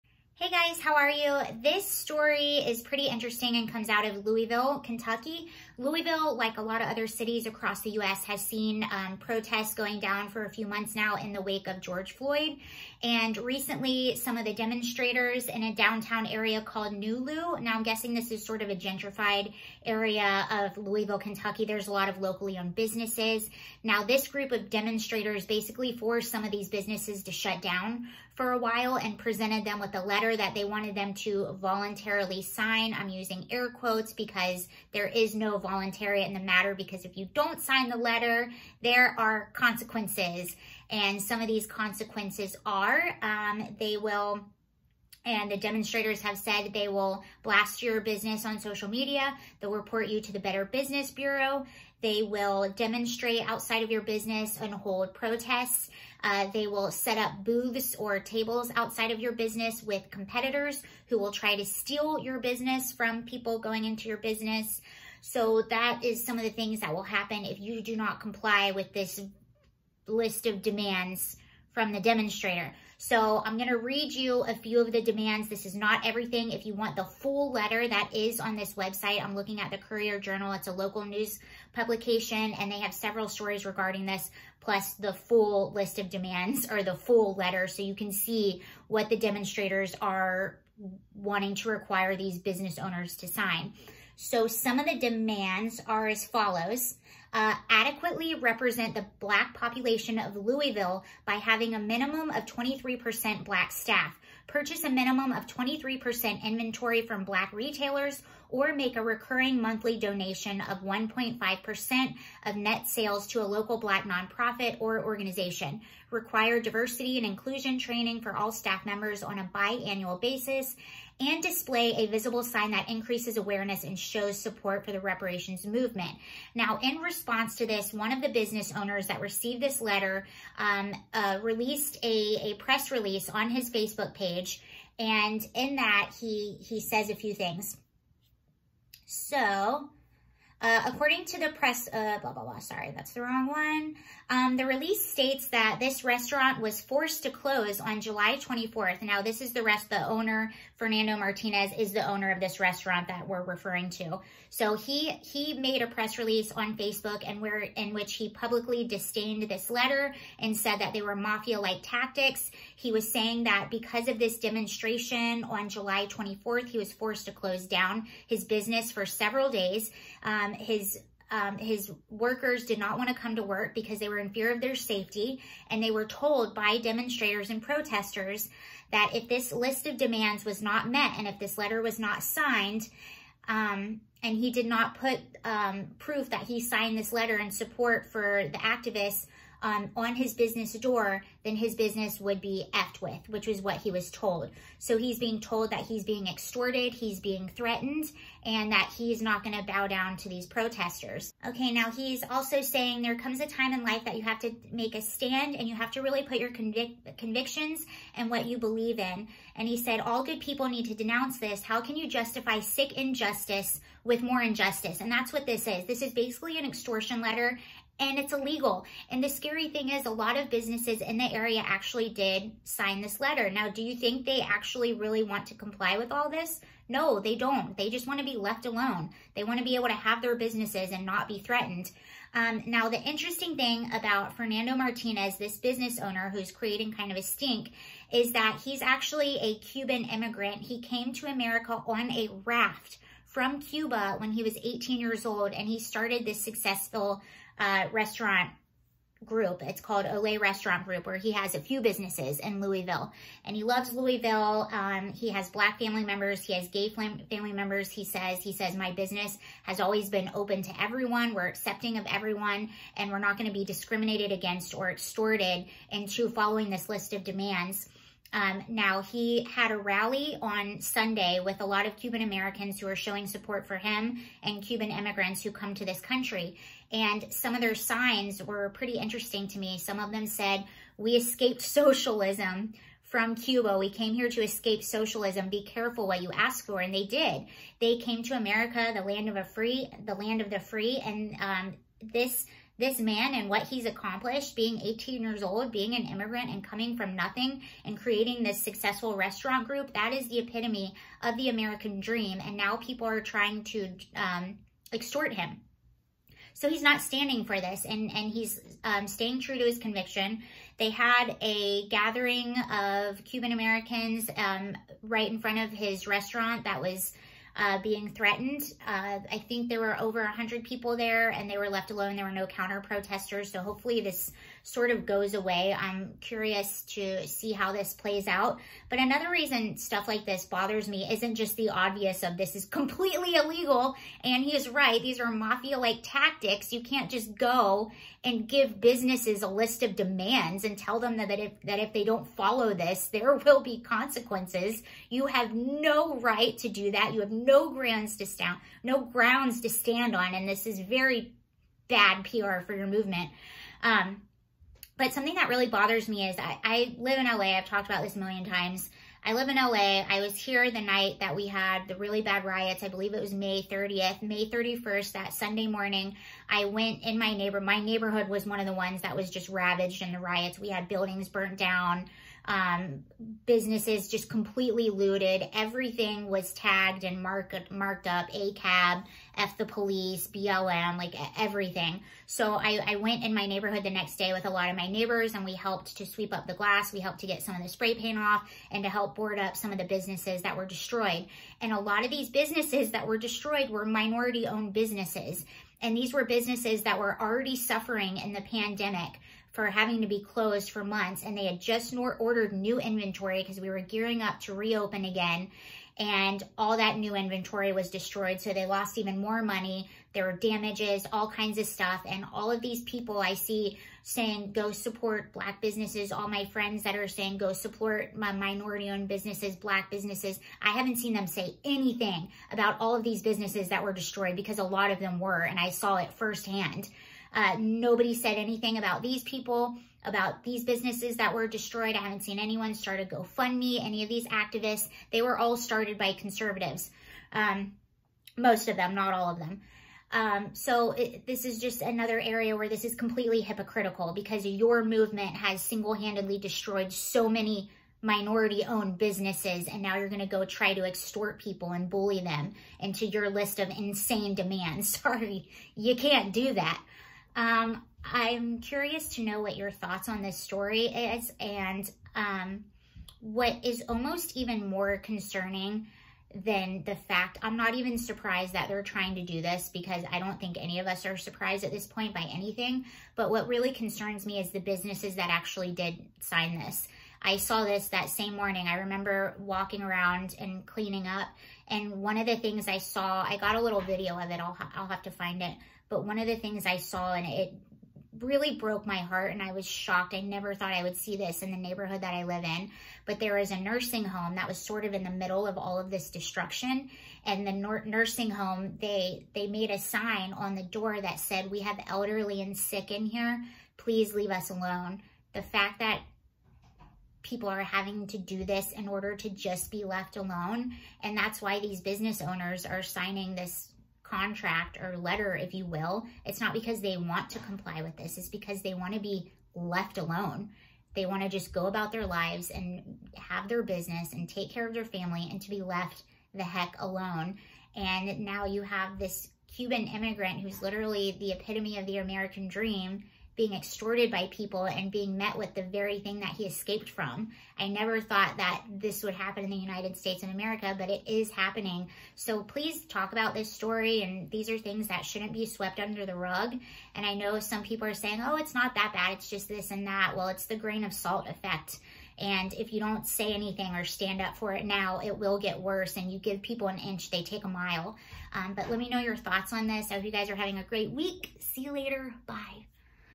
Cuban Community In NuLu Kentucky Stand Behind Business Owner Who Refuses List Of Demands By BLM
Fernando Martinez, a local business owner in Louisville, Kentucky pushes back against demands made by local activists.